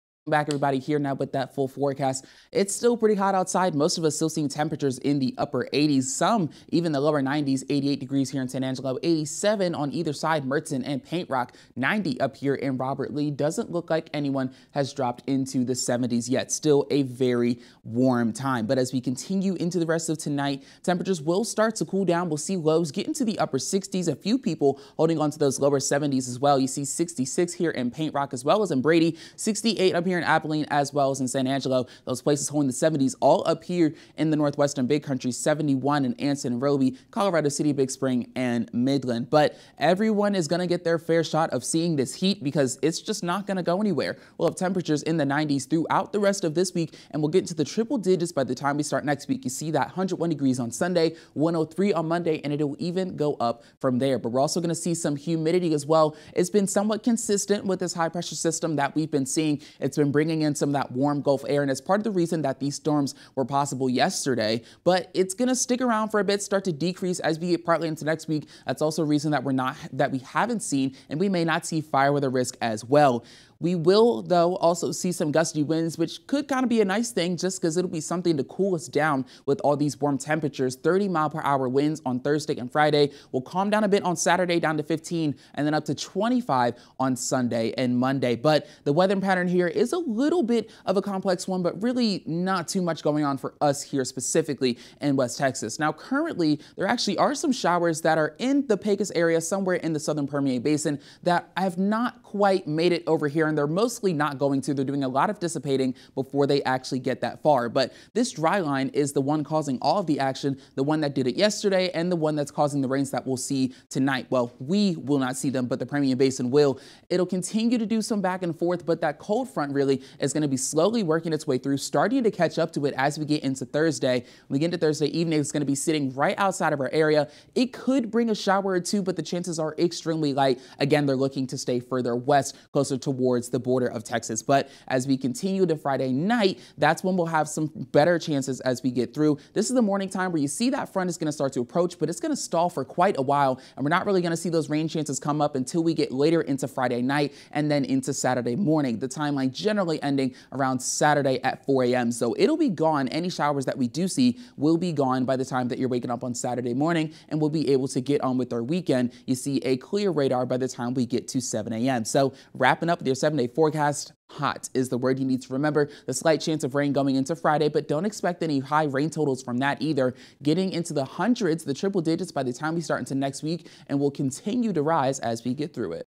Wednesday night forecast 5/11/22
The heat continues for the foreseeable future